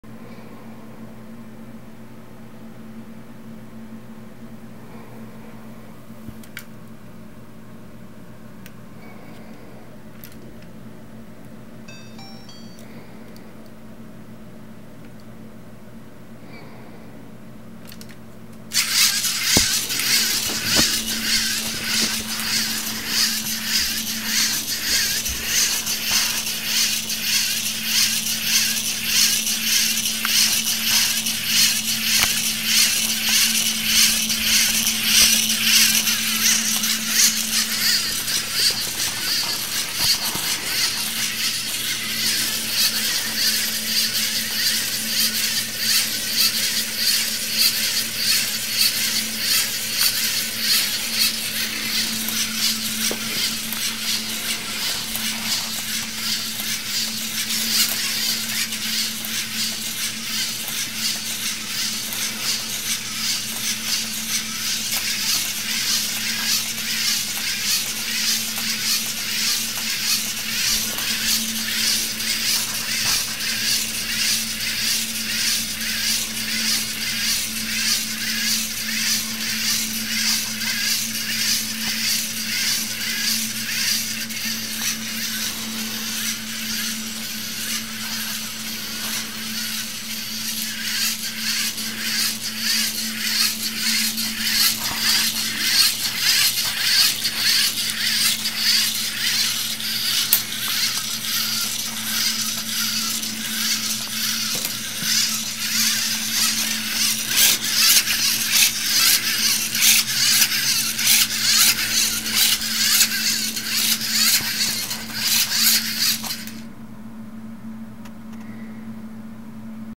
Spiderlike walker with Lego NXT 2.0
My second design attempt at making a "spider" with the NXT.

It has two NXT motors, a color sensor functioning as a light and an ultrasound sensor, with the brick running LeJOS. It runs forward until it detects an obstacle, and then turns away (or at least attempts to). As the end of the video shows, it's hardly an example of great engineering, so I'm back to the drawing board.